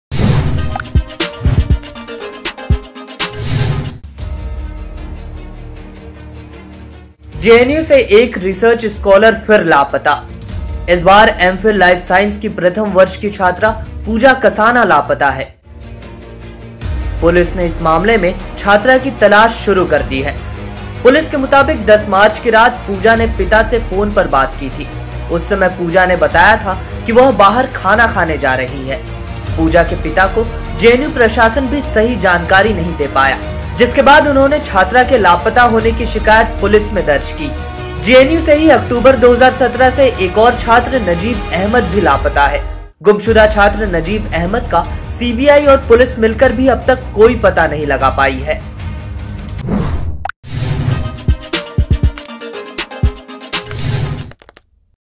JNU Student Missing Case Live News :JNU से एक रिसर्च स्‍कॉलर फिर लापता | JNU Student Went Missing
Najeeb Ahmed, a missing student of JNU (Jawaharlal Nehru University), has been missing months now. Delhi police could not find him yet. Even the CBI is doing research. One more missing student case revealed. another research scholar name is Puja Kasana. She is pursuing MPhil form Jawaharlal Nehru University This is a matter related to a student. Schoolgirl is the first-year student of MPhil Life Science.
JNU से एक रिसर्च स्‍कॉलर फिर लापता | JNU Student Went Missing

जेएनयू यानी जवाहरलाल नेहरू विश्वविद्यालय (JNU) के गुमशुदा छात्र नजीब अहमद को लापता हुए अब महीनों हो चुके हैं. पुलिस अभी तक उसका पता नहीं लगा पाई. यहां तक की सीबीआई भी खोजबीन ही कर रही है. जेएनयू से अब एक अन्य रिसर्च स्‍कॉलर के लापता होने का मामला सामने आया है. यह एक छात्रा से जुड़ा हुआ मामला है. छात्रा एमफिल लाइफ साइंस की प्रथम वर्ष की छात्रा है.